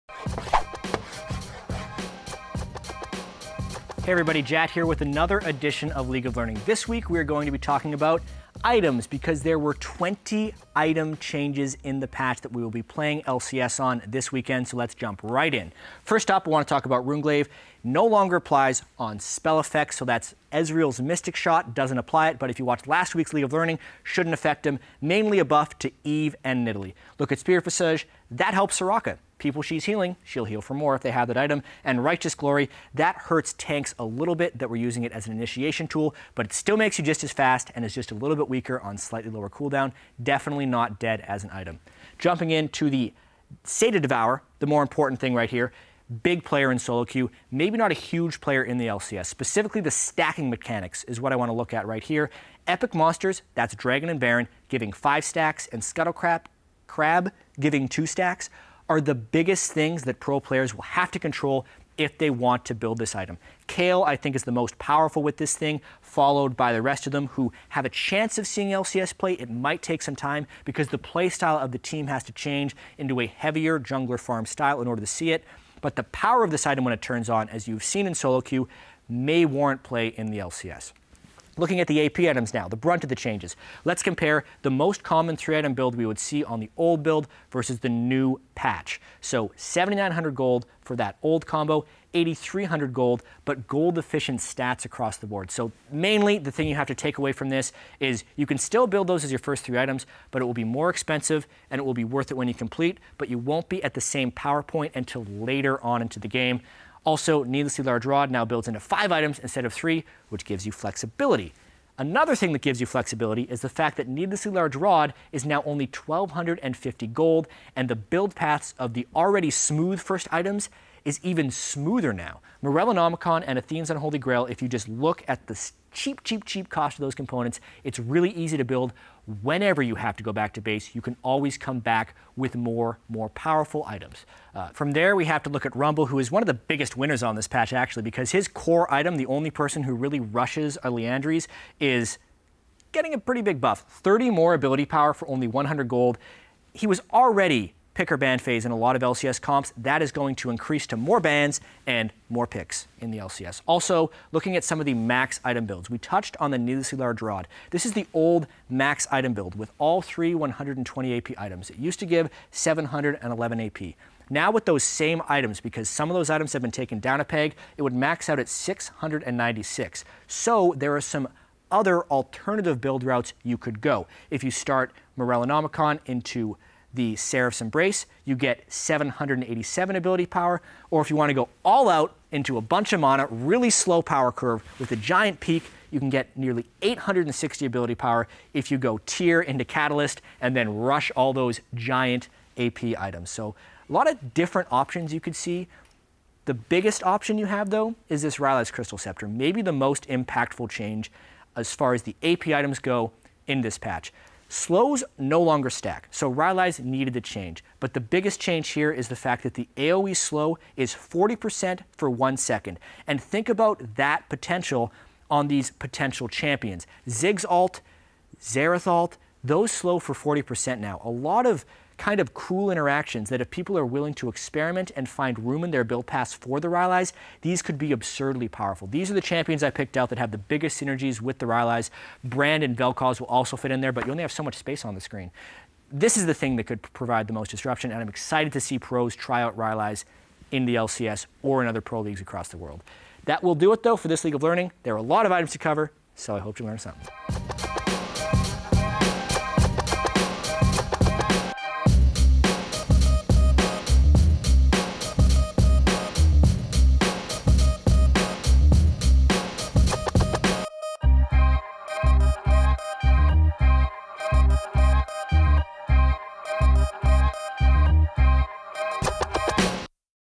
League of Learning: AP Items
Jatt breaks down the new AP items and build paths in patch 5.13. Get your fill of needlessly large learning.